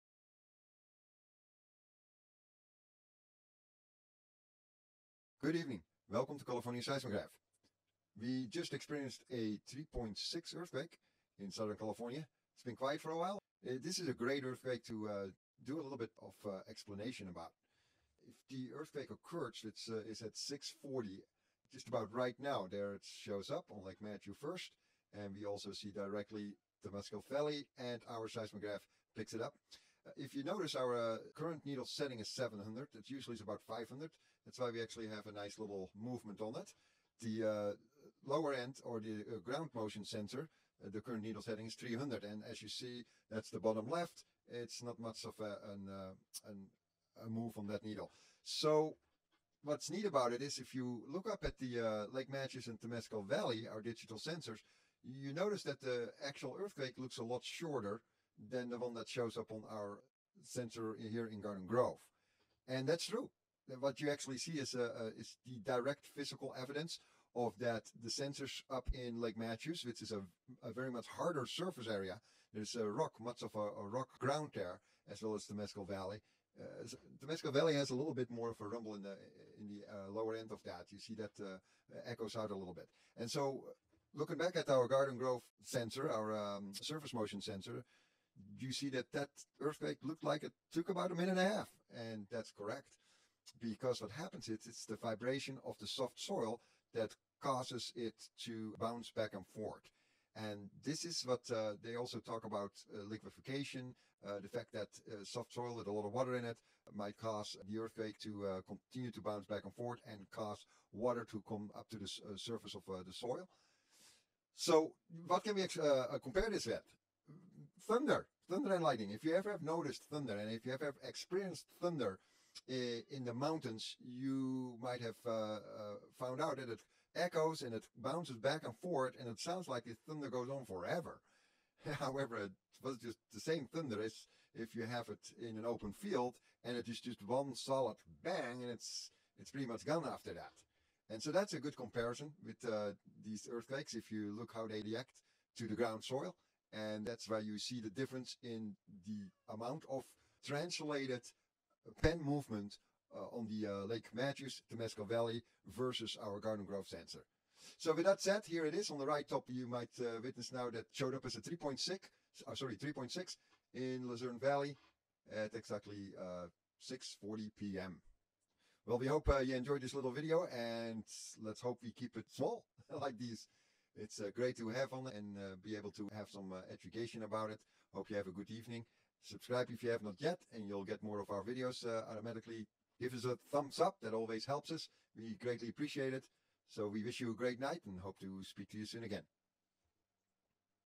3.6 Earthquake Lucerne Valley, CA.
A minor earthquake occurred at 6:40:12 PM (PST) on Tuesday, January 29, 2019. The magnitude 3.6 event occurred 5 miles ESE of Lucerne Valley, CA. The hypocentral depth is 0.3 miles deep.